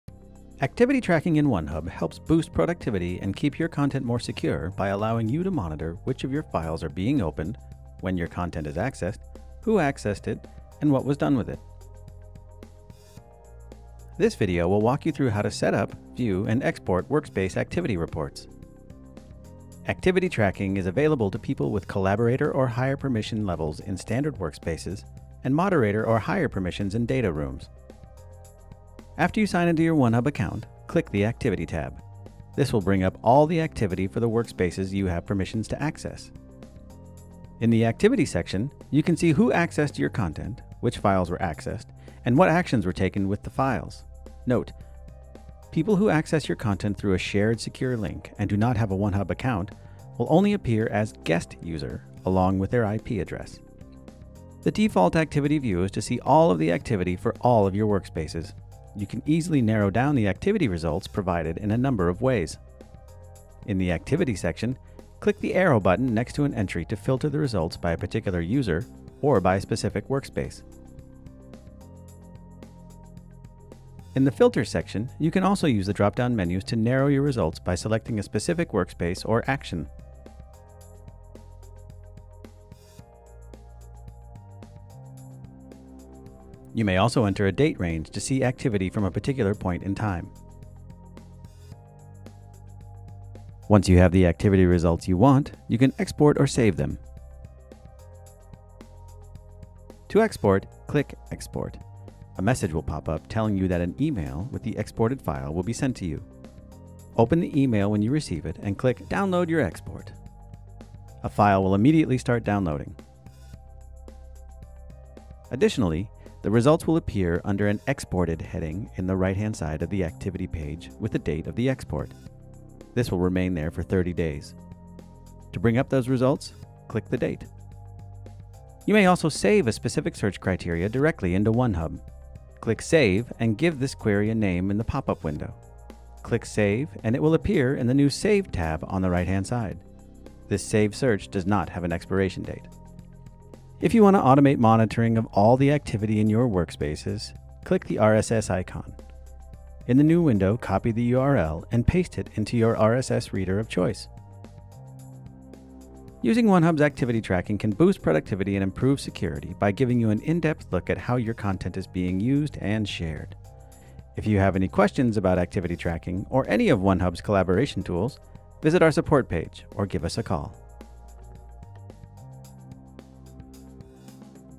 Onehub Tutorial: Using Activity Tracking
Activity Tracking in Onehub lets you see who has accessed your content, when it was accessed and what was done with it. This video will walk you through the steps of setting up Activity Tracking in your Onehub account.